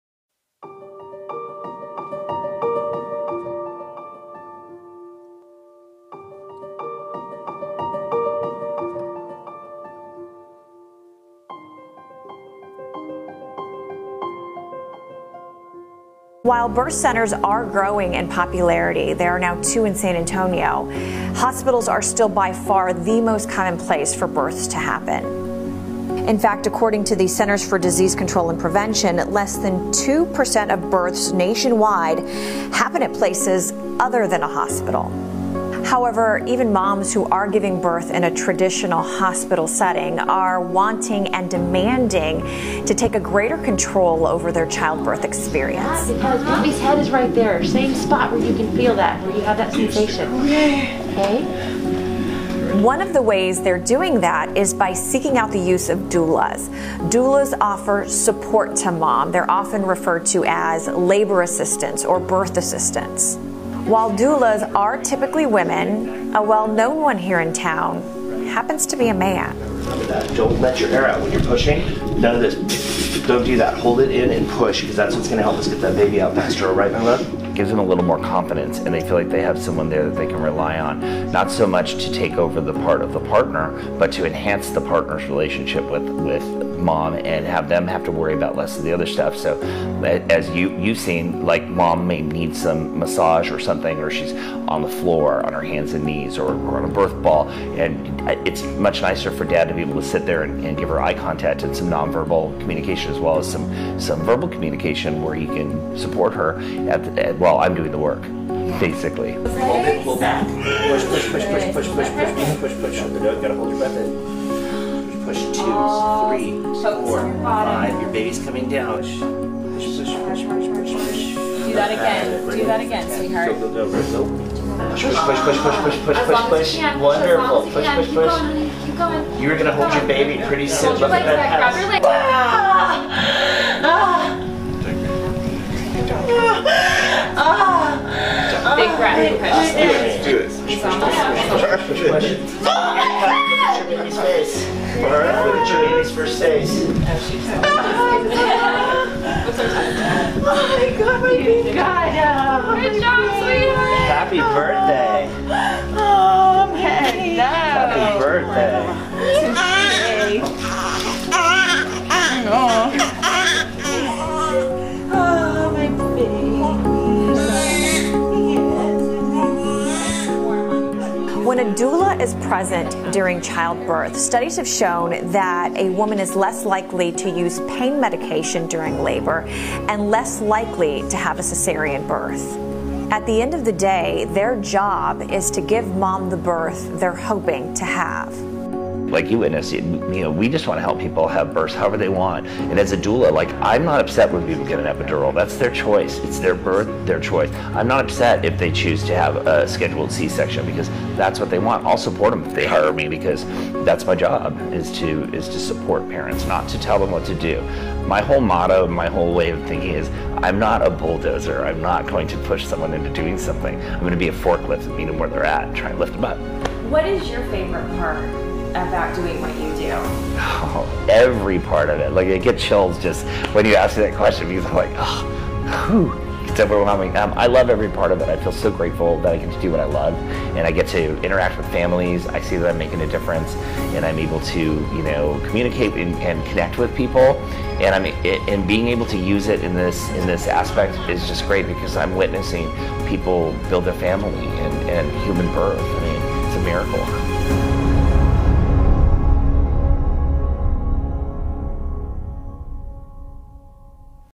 Up Close: Brian the Birth Guy Segment
Having worked with over 19,000 expectant families, Brian has taught many childbirth classes and currently teaches the incredibly popular Dad’s childbirth class “Rocking Dads” and couples birth class “Facilitating Fearless Birth”.